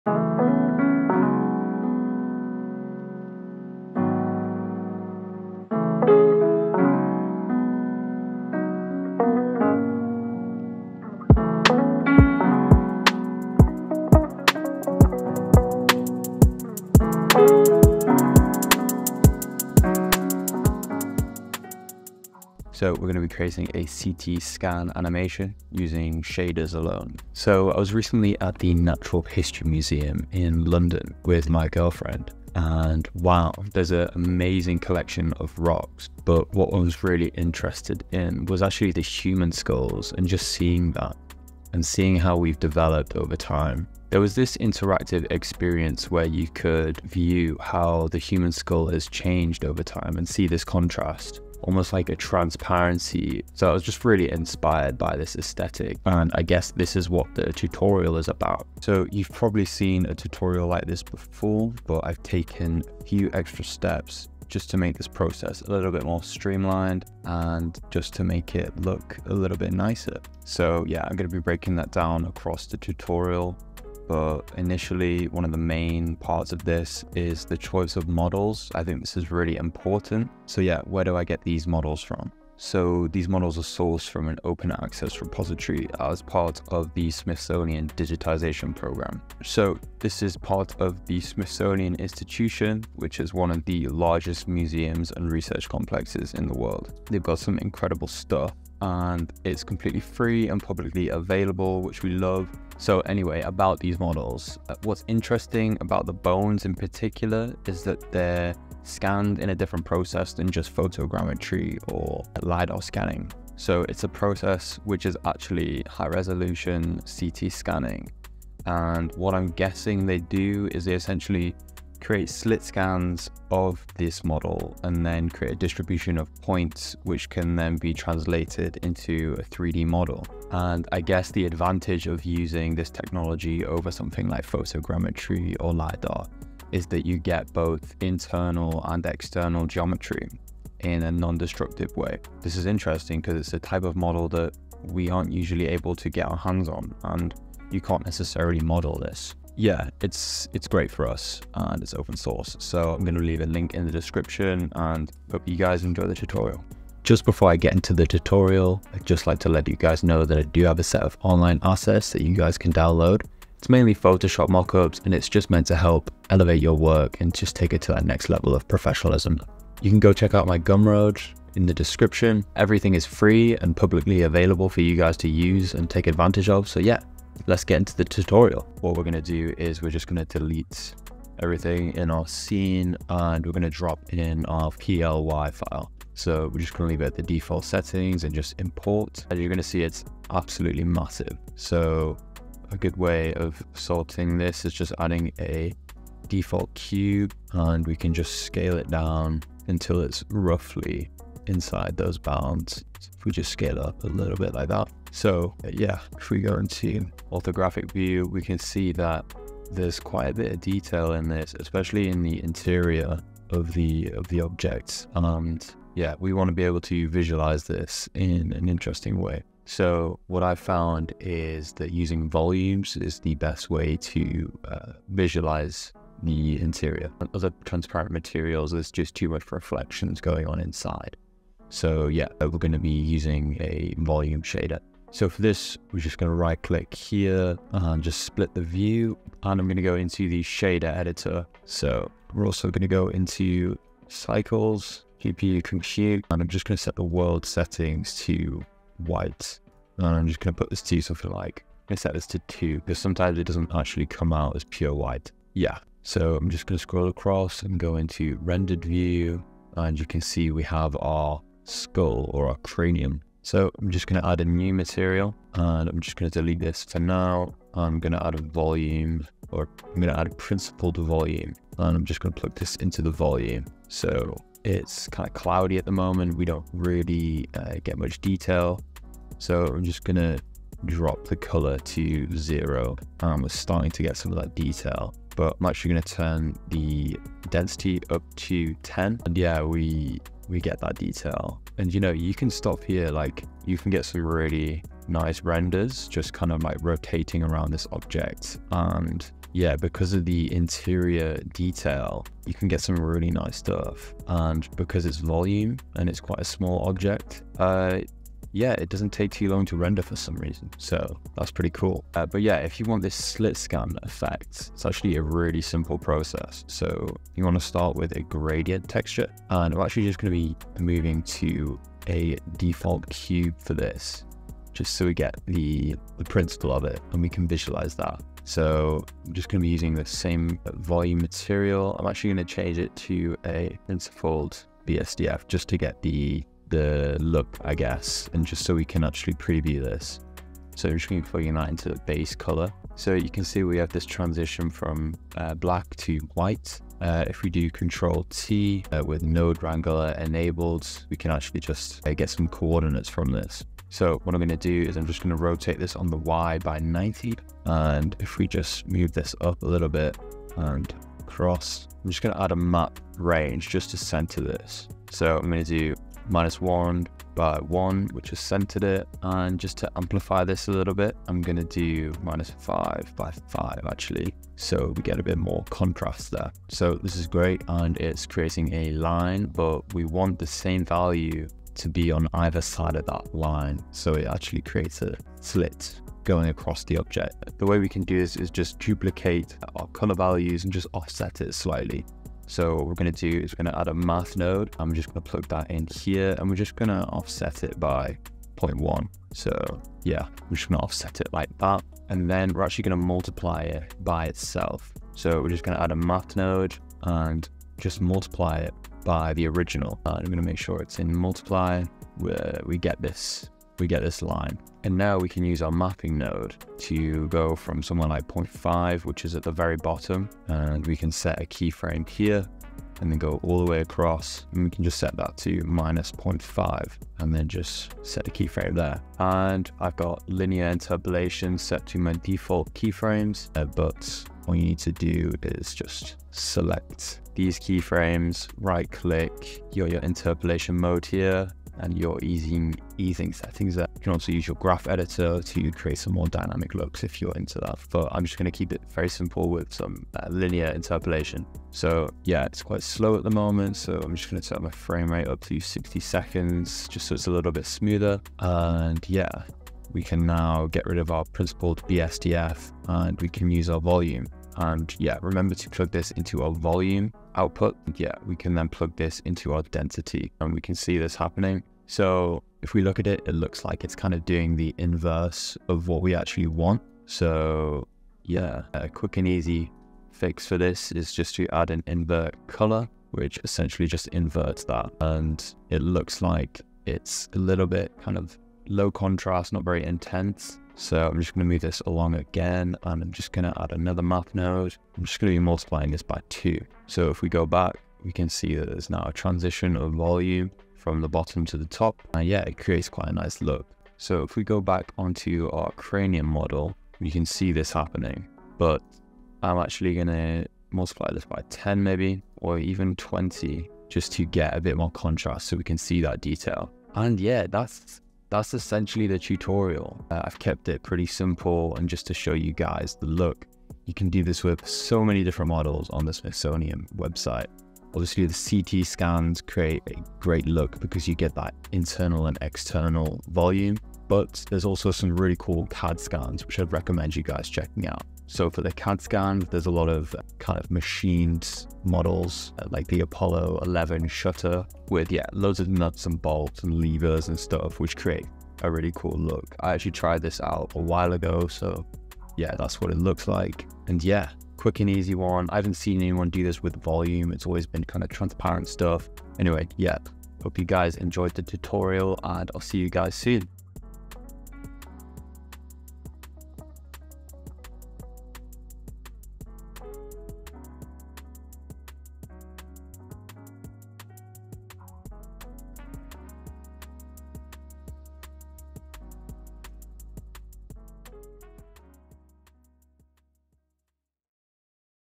Crazy Easy CT Scan Animation with Blender
0:00 - Introduction and Inspiration
0:54 - Overview of the Tutorial
1:14 - Importance of Model Choice
1:37 - Smithsonian Digitization Program
2:11 - CT Scanning vs. Photogrammetry or Lidar
2:33 - Unique Features of High-Resolution CT Scans
2:50 - Link to Online Assets
3:12 - Importing and Scaling the Model
3:35 - Preparing the Scene for Visualization
4:00 - Using Volumes to Visualize Transparency
4:20 - Shader Setup in Blender
4:36 - Adjusting Density for Better Detail
5:00 - Gradient Texture Setup
5:20 - Creating a Default Cube for Preview
5:40 - Using Math Nodes for Effects
6:17 - Adding Keyframes for Animation
7:14 - Smoothing Animation with Linear Interpolation
7:40 - Final Adjustments to Frame Rate and Output
8:22 - Adding Color to Enhance Contrast
8:56 - Applying the Effect to the Cranium Model
9:10 - Conclusion